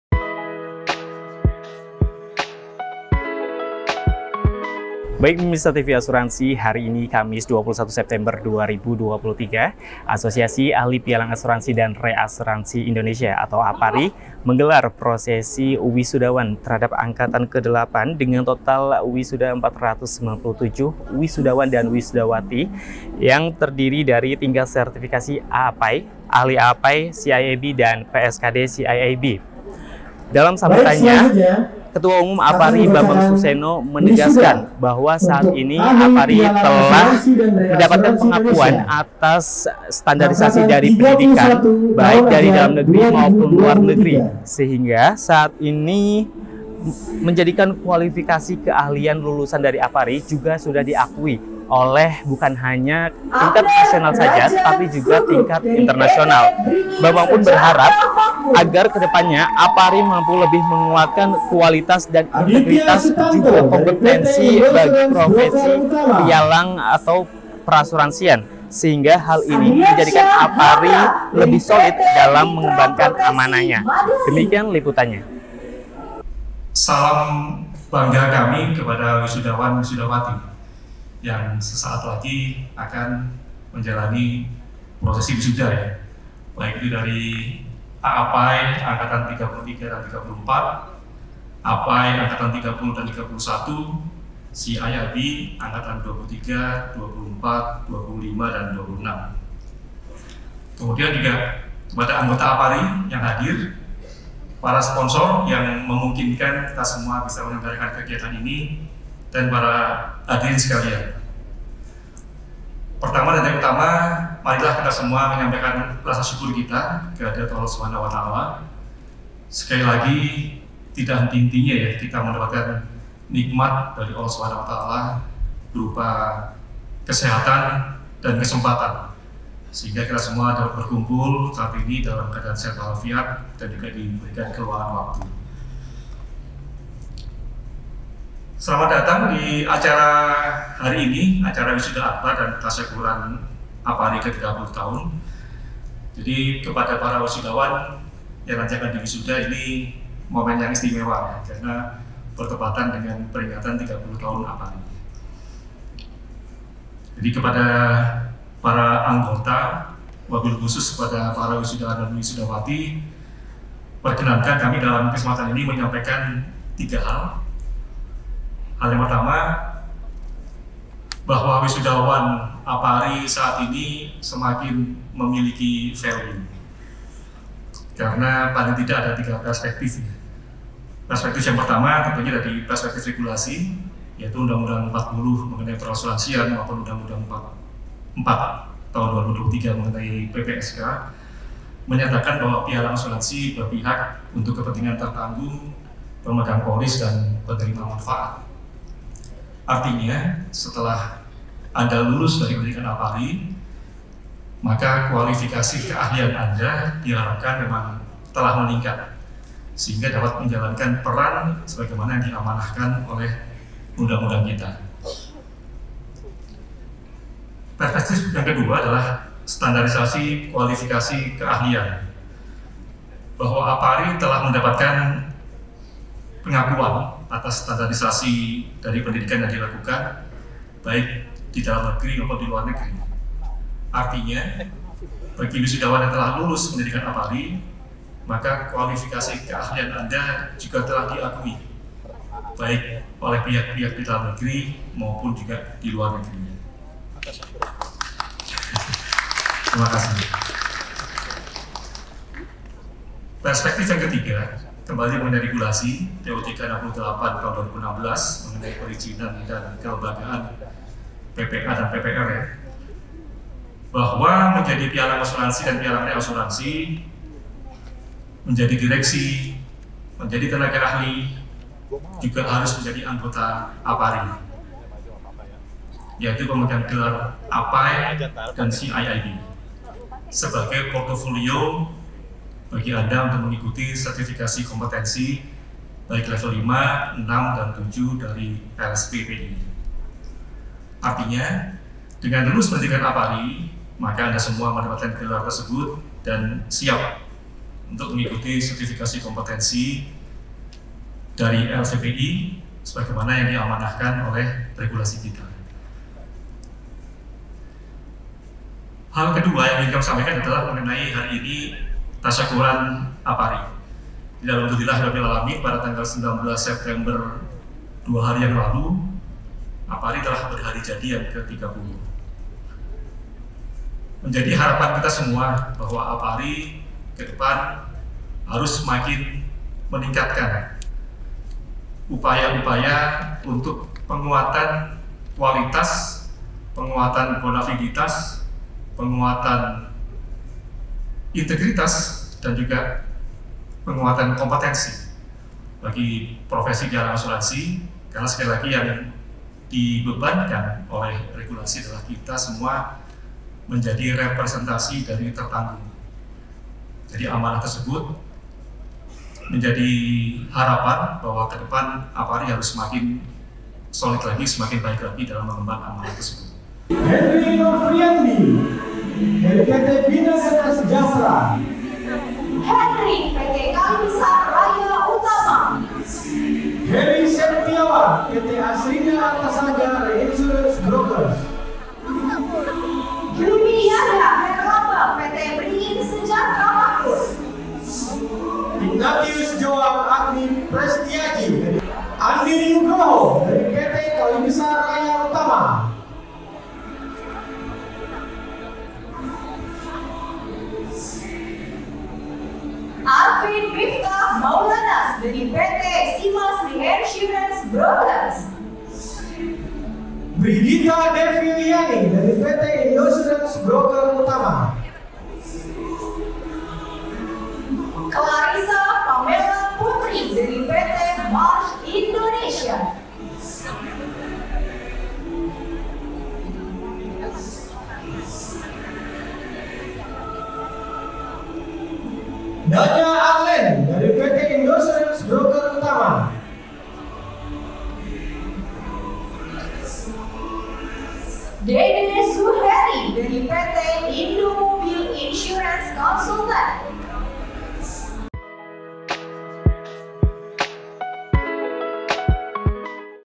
APARI Gelar Wisuda 8 Angkatan
Asosiasi Ahli Pialang Asuransi (APARI) kembali menggelar wisuda akbar terhadap delapan angkatan, dengan jumlah wisudawan/wati sebanyak 497 yang terdiri dari sertifikasi tingkat AAPAI, APAI, CIIB, dan PSKD CIIB.